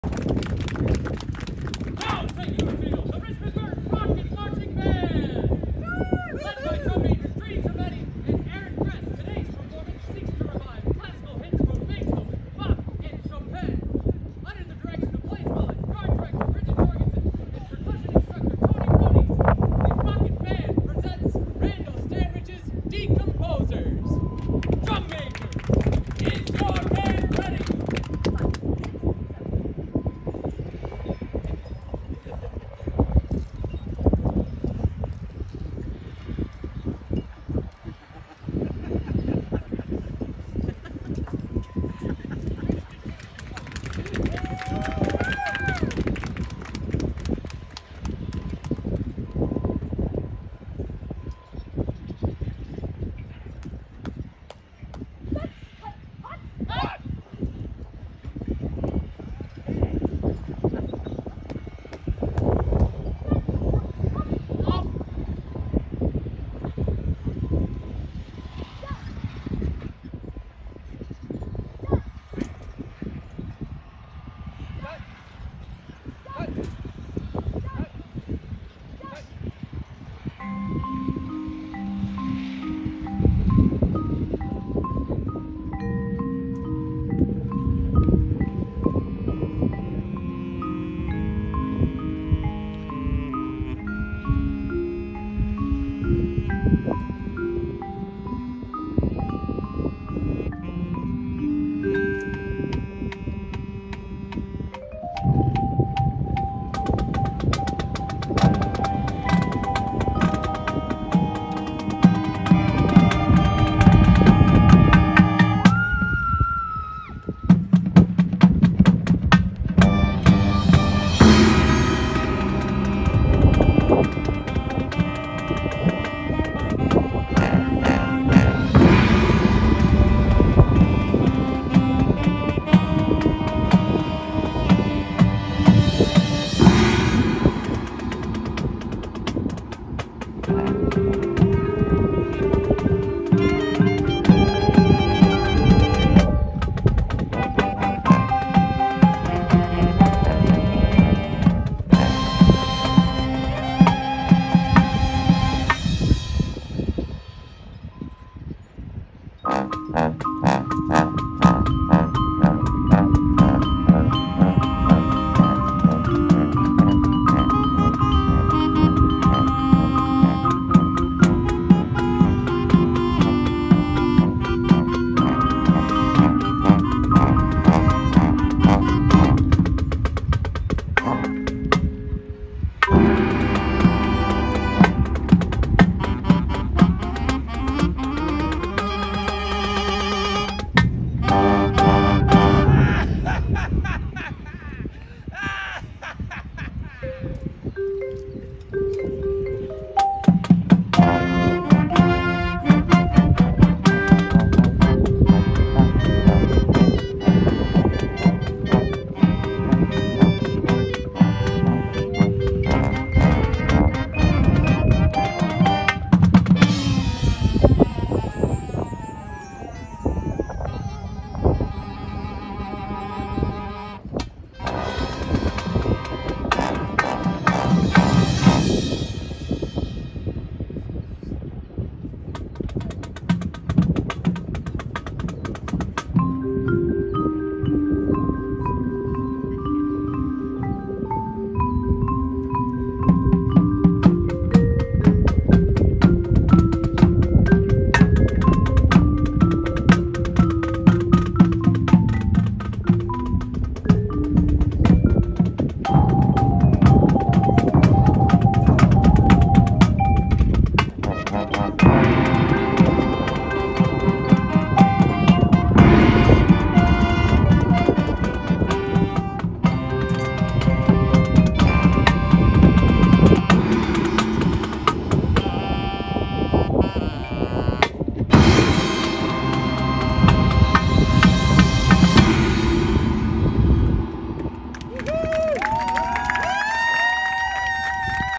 Kaylee’s HS Marching Band Performance
After a week of 9hr practice days, the HS marching band is ready to perform.